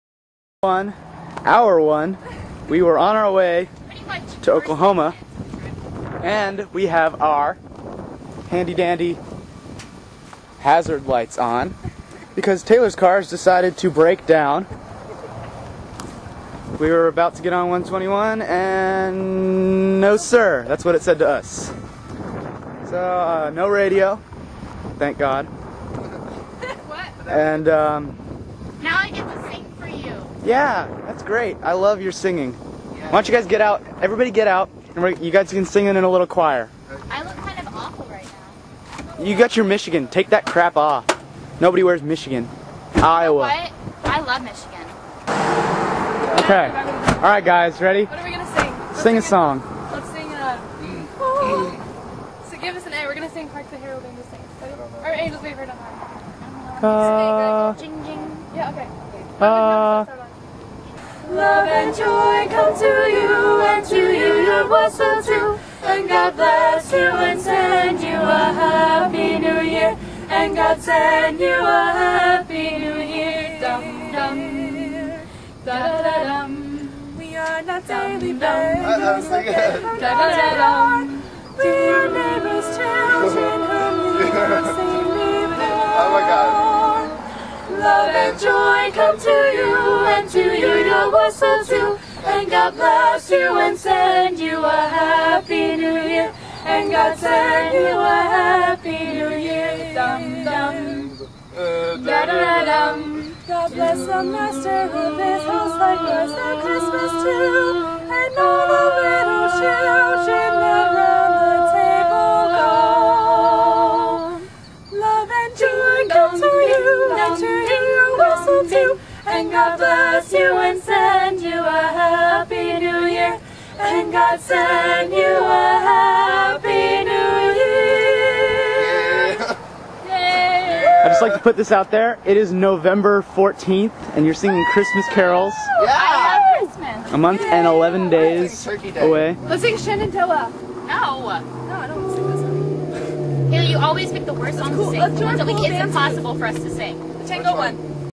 2009 Nov Hmm Car Broke Down So What To Do.wmv
Friends break out the Christmas cheer by caroling while waiting for help on the side of the road.